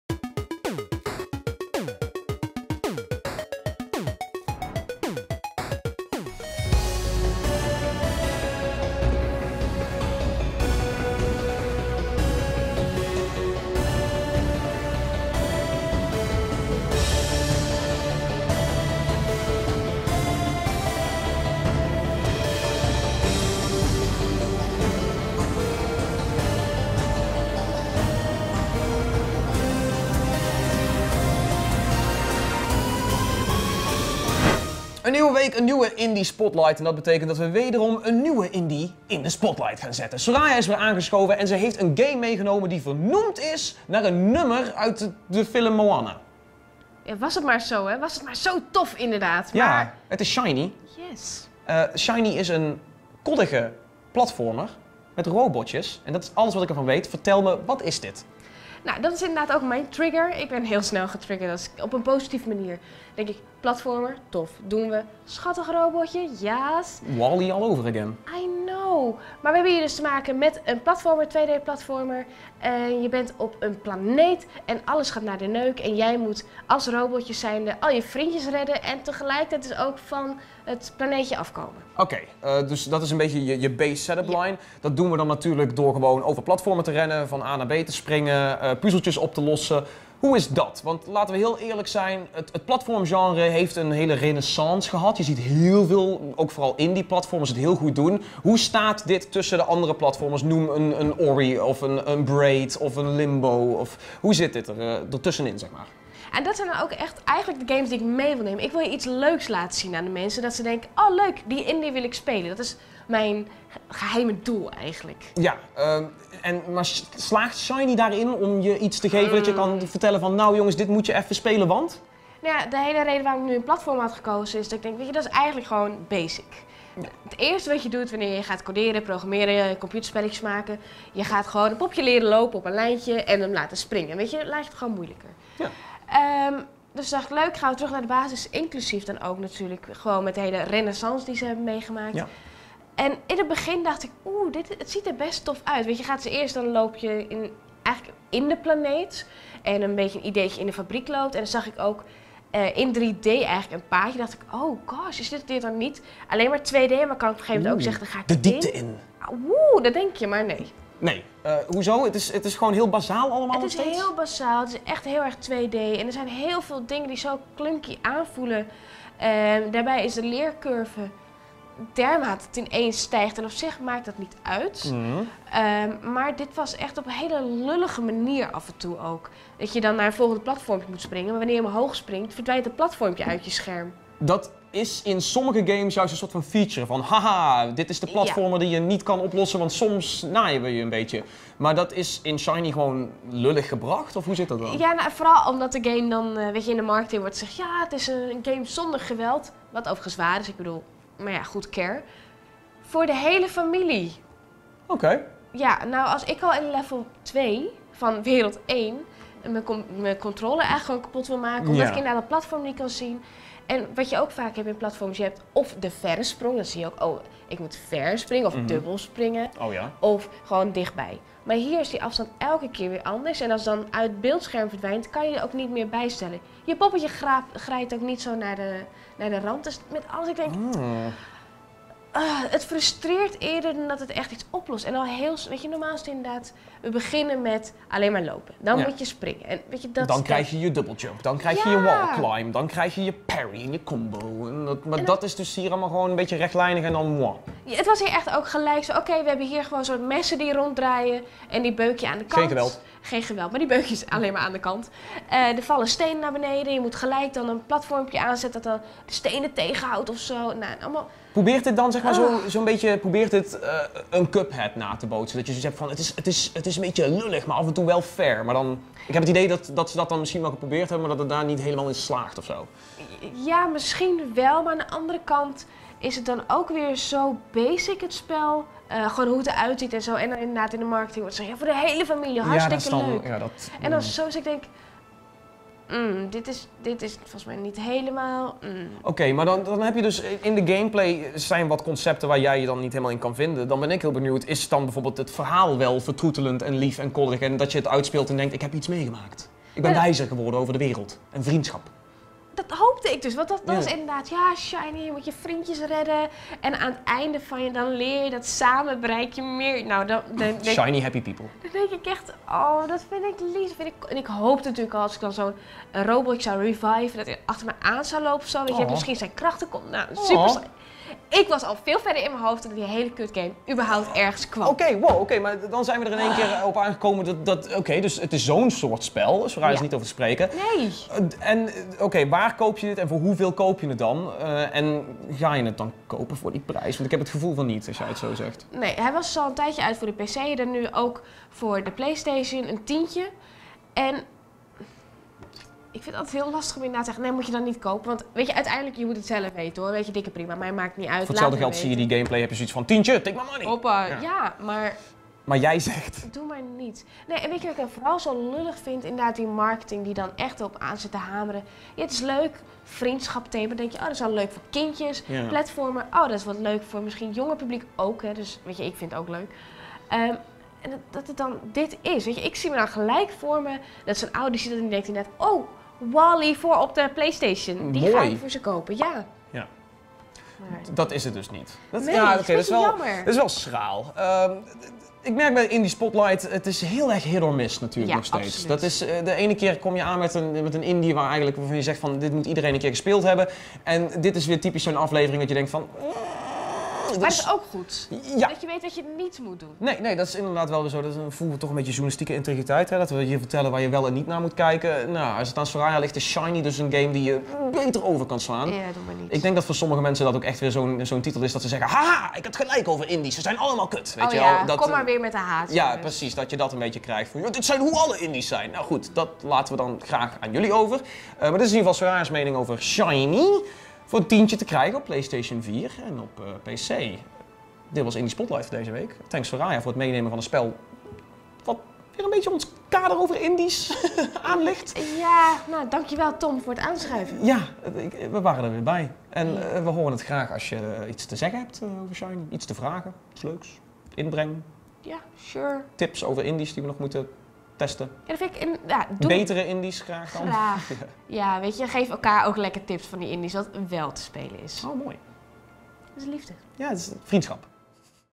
Weet Shiny te kunnen schitteren tussen de platformers? | Indie Spotlight
Van SOEDESCO en het Braziliaanse ontwikkelbedrijf 1C Games verschijnt nu Shiny op PlayStation 4, Xbox One en pc. Een koddige platformer met een eigen gezicht, maar is dat genoeg om onze indie-dame Soraya te overtuigen? Tom vraagt haar de oren van het lijf voor een nieuwe Indie Spotlight...